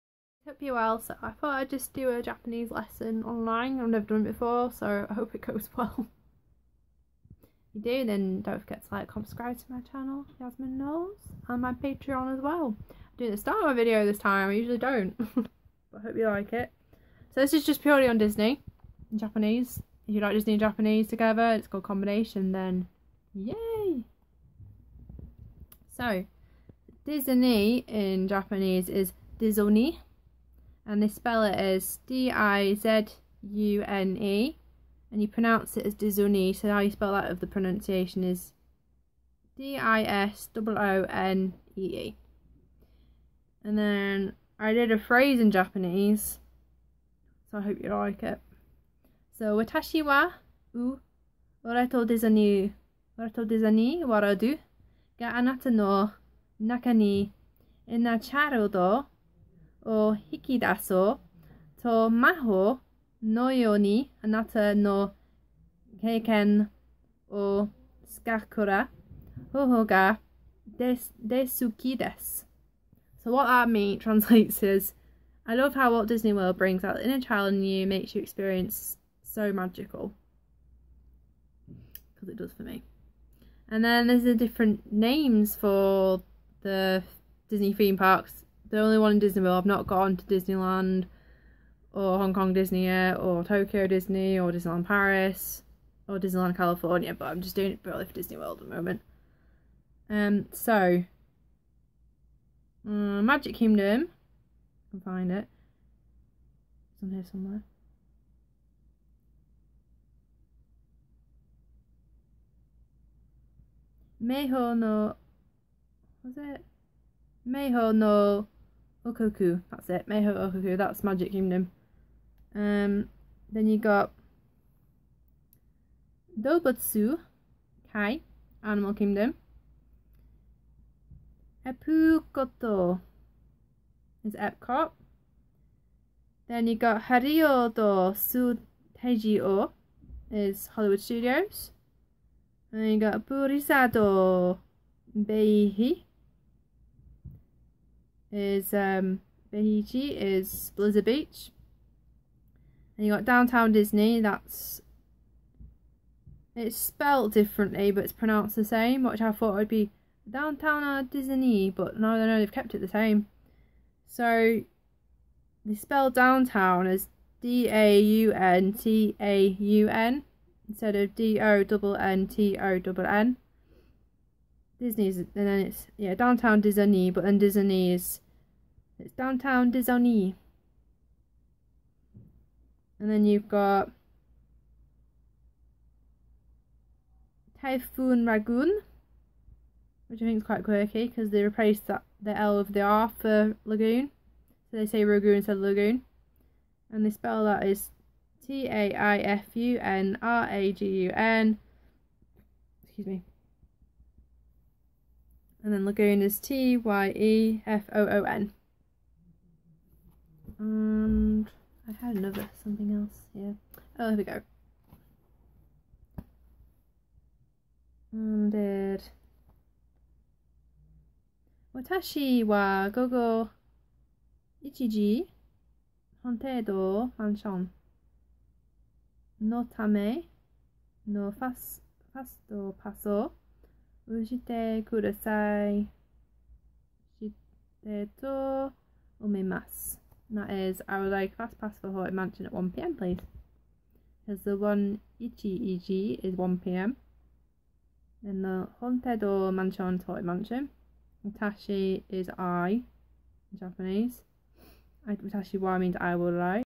Japanese: Disney Lesson
Hey guys here is my lesson on some phrases on Disney and Walt Disney World. If you want me to do more Japanese online lessons let me know.

Sorry it cut off at the end no idea why my phone did that lol 😂 XD